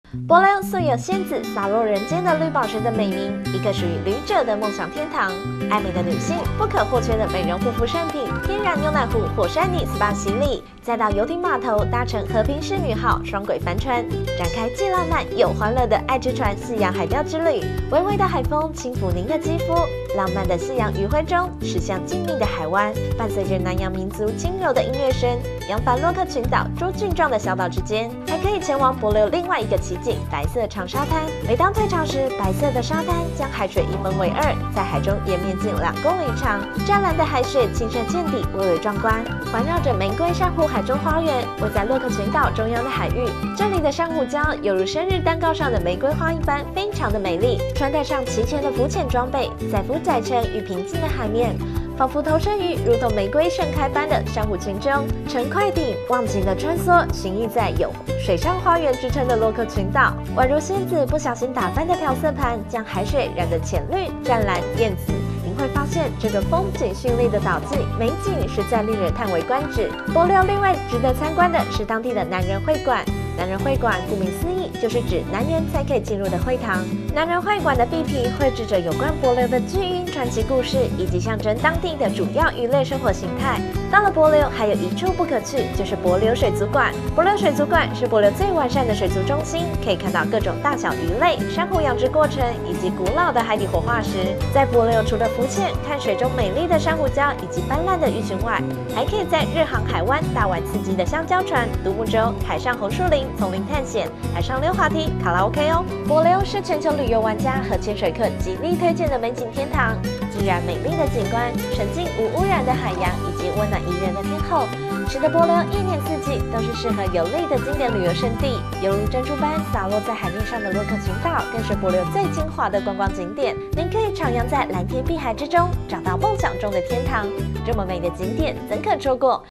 享受不一樣的夏天，到帛琉浮潛做SPA
~~飛往帛琉尋找浪漫，更多旅遊選擇。
想做個天然SPA，就要到帛琉的牛奶湖火山泥享受一下，不只如此，帛琉的海底世界更是美不勝收。